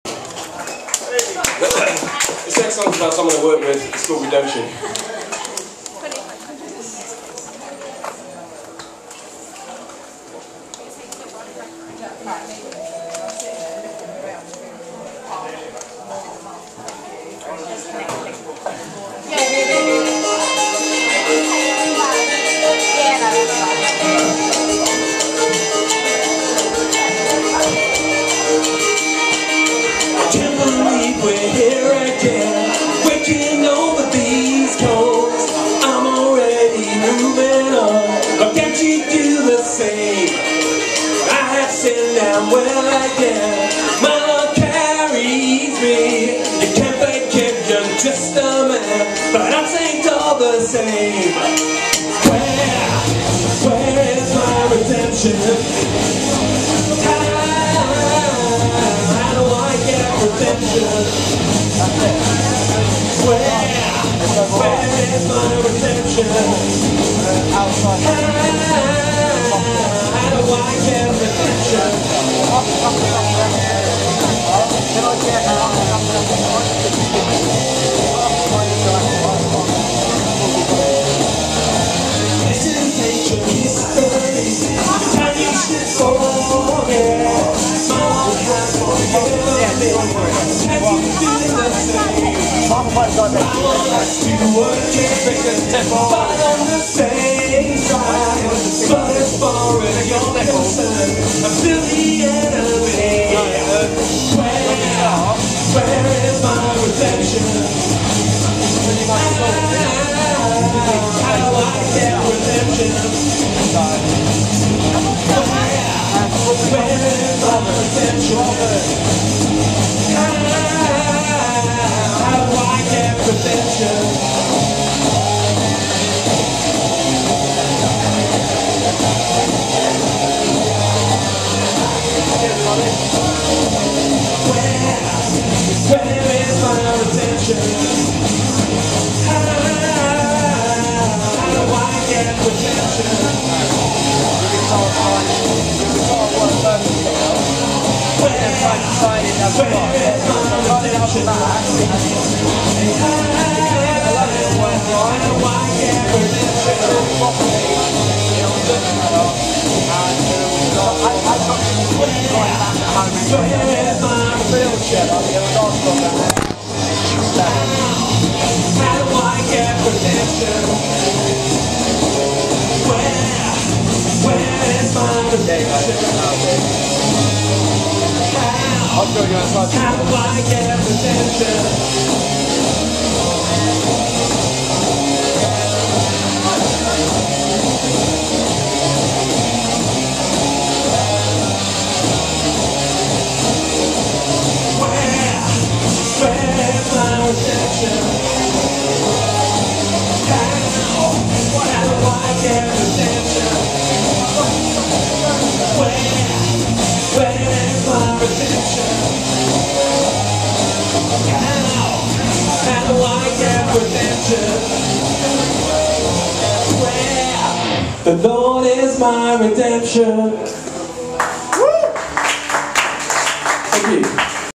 Redemption at the Red Roar Festival
100 robots performing Redemption at the Red Roar Festival in Gravesend